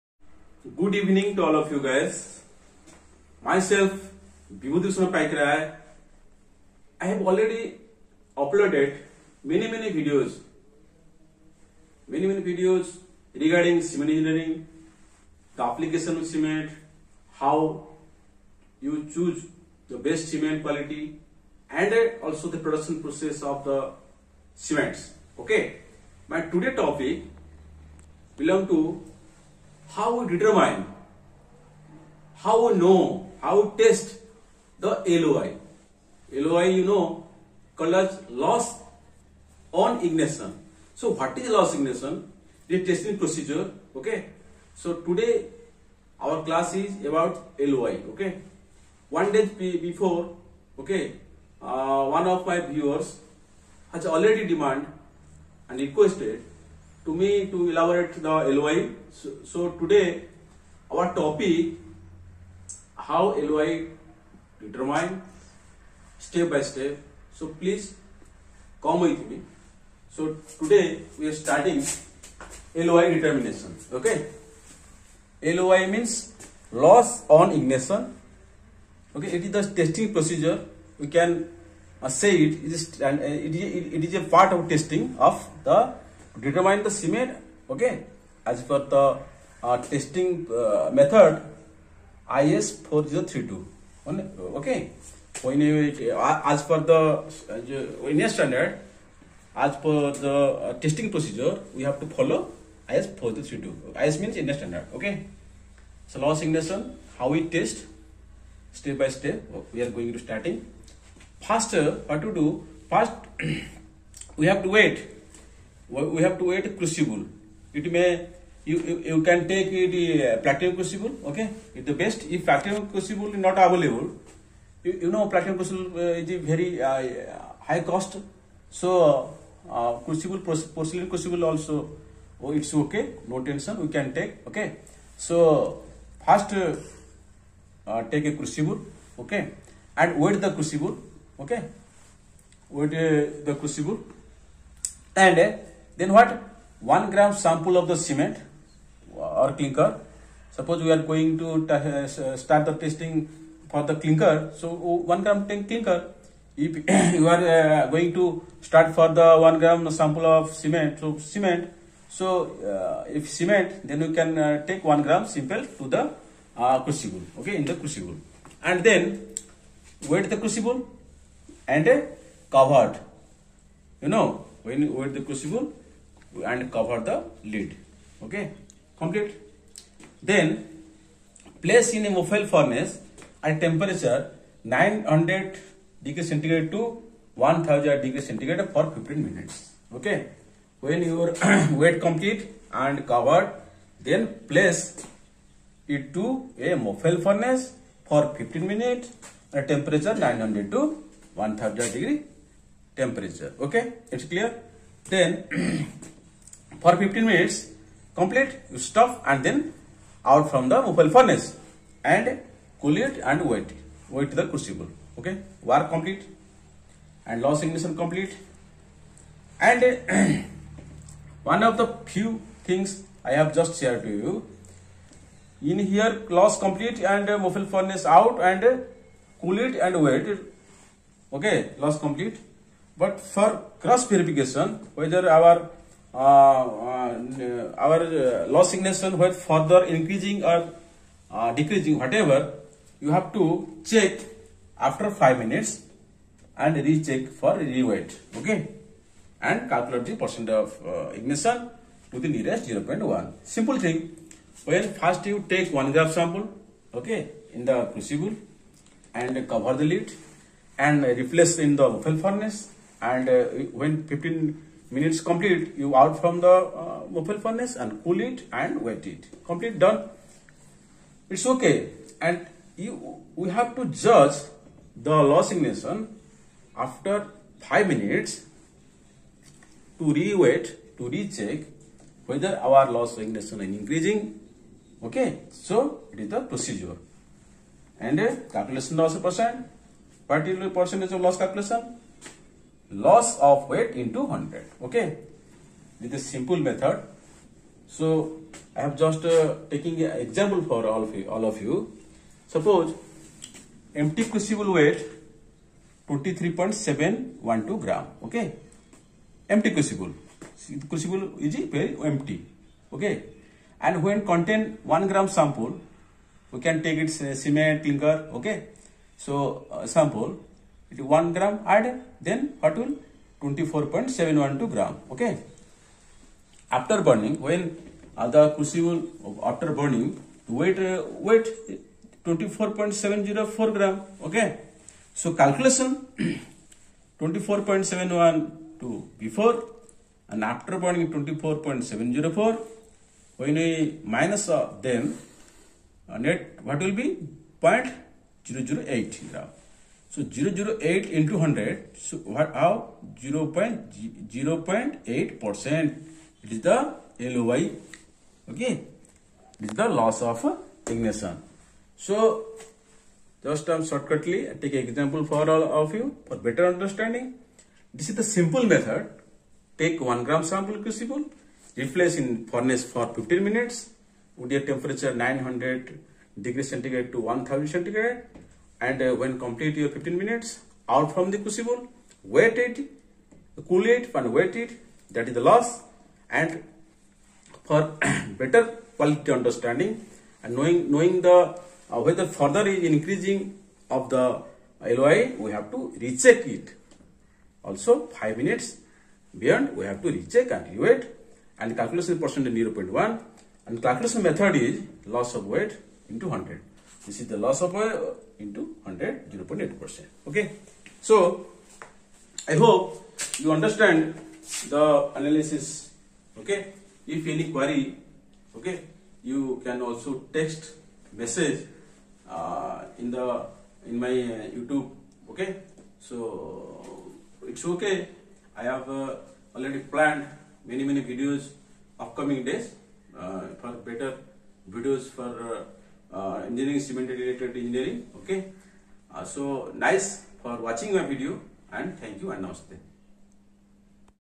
L.O.I (Loss on Ignition ) | Determination for L.O.I Of Cement As Per IS: 4032 | L.O.I Test Method
L.O.I (Loss on Ignition ) | Determination for L.O.I Of Cement As Per IS: 4032 | L.O.I Test  Method | What is loss on ignition | loss on ignition is a method estimates weight of sample before heating minus the weight after heating and Calculate % of loss of ignition | The Cement industry utilise the LOI method to determine the water and /or carbonation in the Cement as these reduce the Quality |